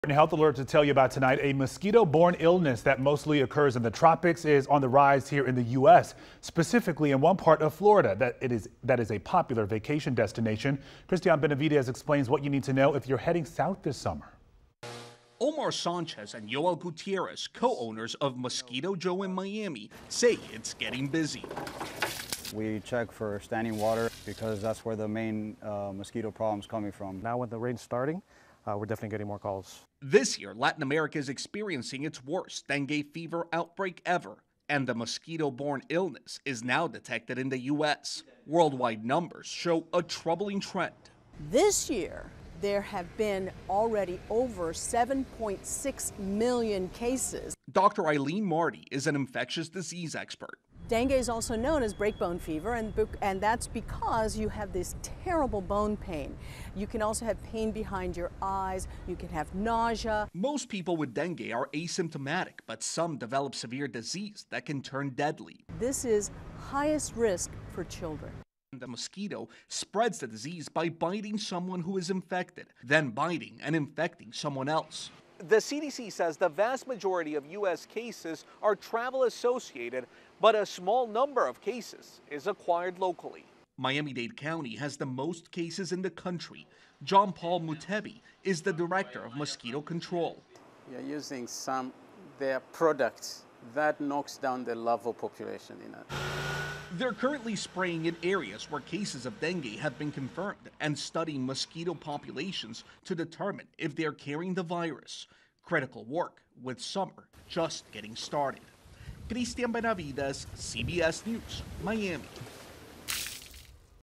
Mosquito-borne illness on the rise in the U.S.
THIS YEAR, LATIN AMERICA IS EXPERIENCING ITS WORST DENGUE FEVER OUTBREAK EVER AND THE MOSQUITO BORNE ILLNESS IS NOW DETECTED IN THE U.S.